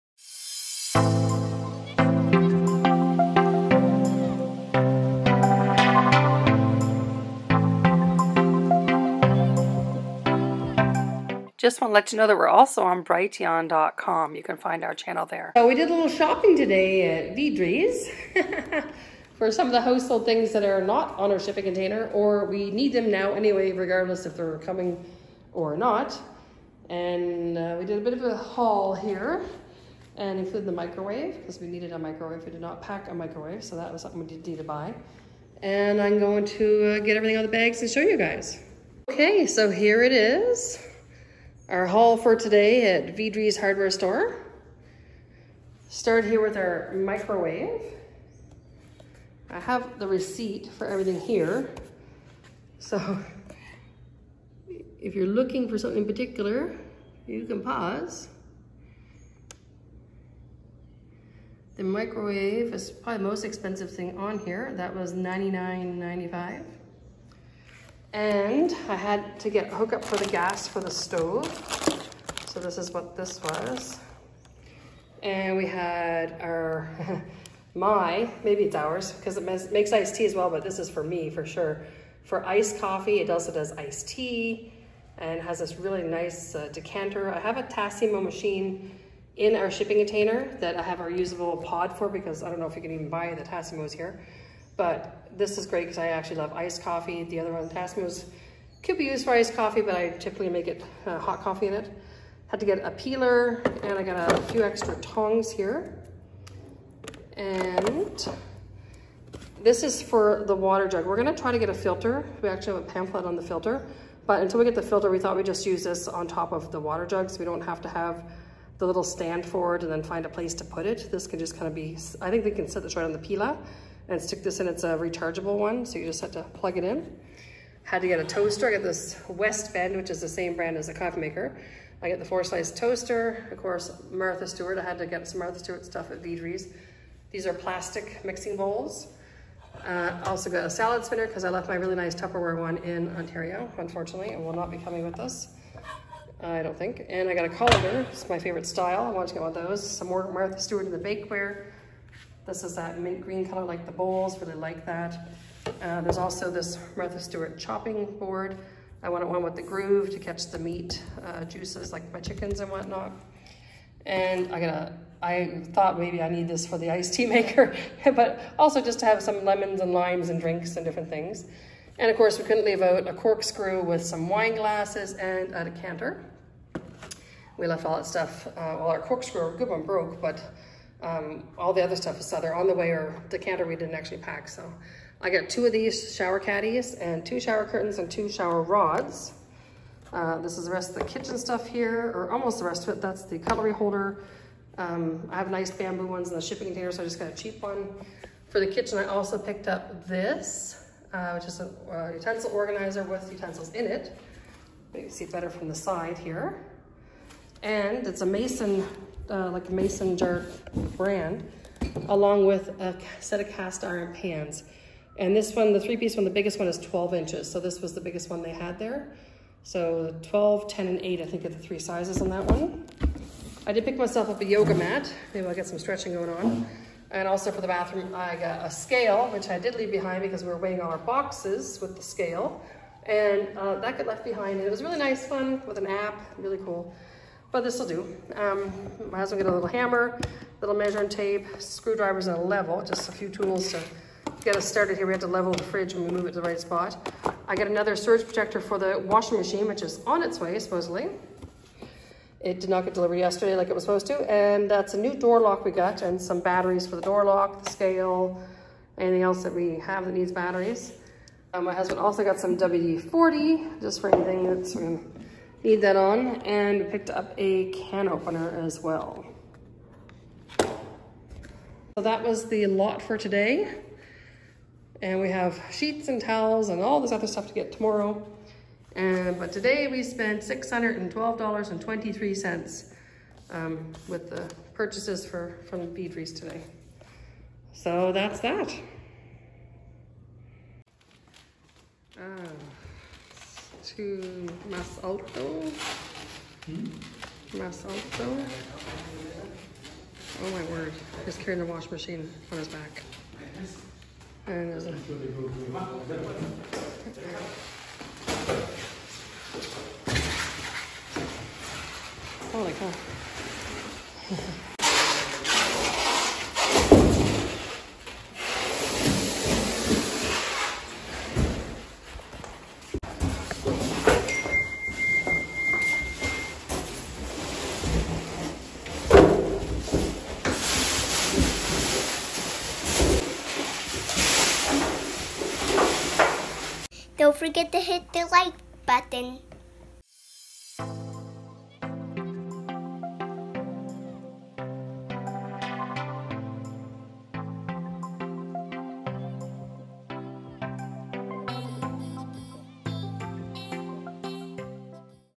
Vidri's haul and Washing machine delivery
Today went to Vidri’s to get a few things that we needed to set up our new home in El Salvador.  We also had our washing machine delivered by La Curacao as they couldn’t get it to us yesterday, you wouldn’t believe how they got it in, stay till the end to see it.